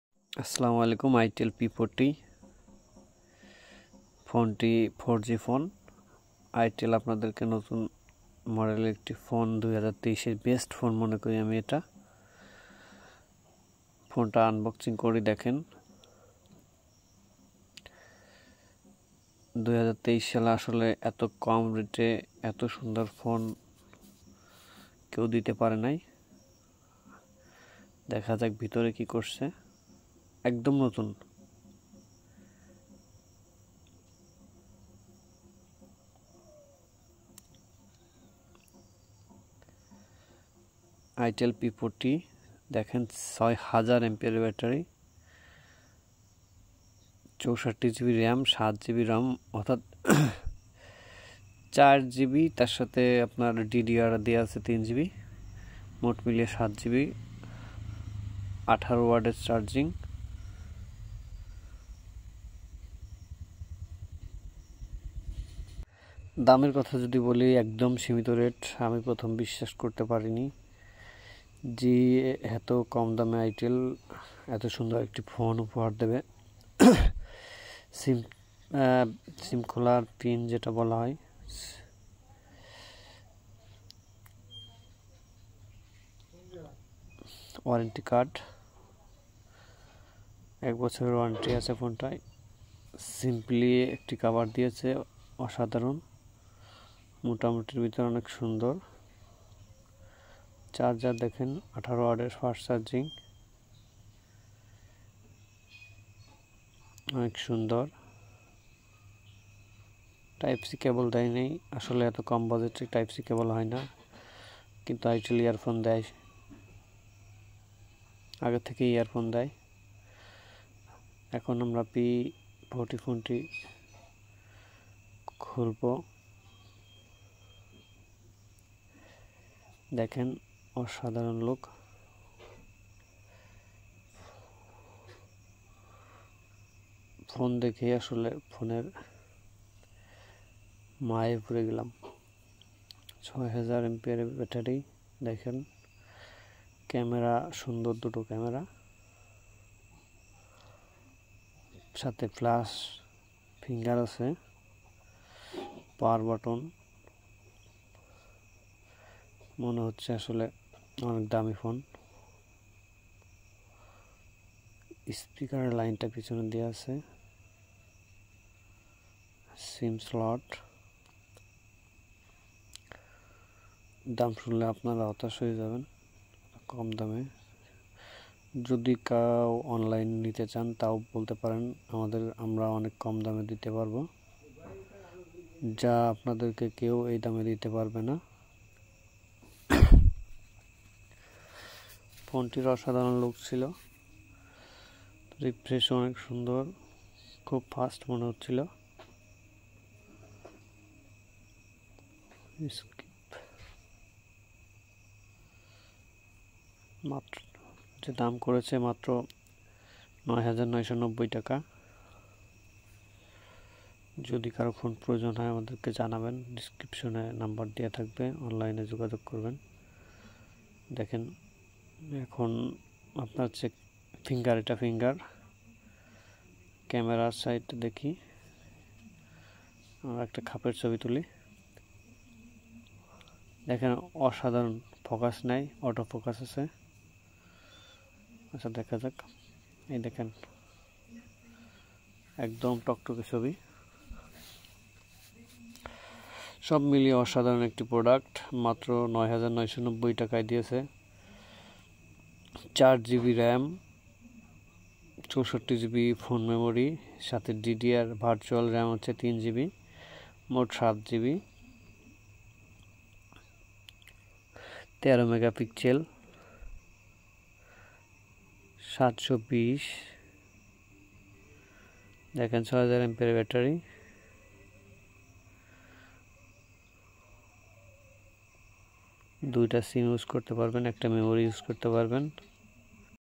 ৮৯৯০ টাকায় স্মার্ট ফোন 7GB RAM ৬০০০ এম্পিয়ার ব্যাটারী
itel P40 Review BANGLADESH

দাম👉 ৬৪+৪ জিবি (৯,৯৯০ 🔥 টাকা), ৩২+৩ জিবি (৮,৯৯০ টাকা)।

P40 Specification :
- 6.6" HD+ Waterdrop FullScreen
- 6000mAh Mega Battery
- 13MP AI Dual Rear Camera with Flash
- 5MP Front Camera
- 64GB ROM + 4GB RAM  /  32GB ROM + 3GB RAM
- Sc9863A Octa-core
- Fingerprint Sensor + Face Unlock
- Dual 4G
- Android 12 (4GB) / Android 12 Go (3GB)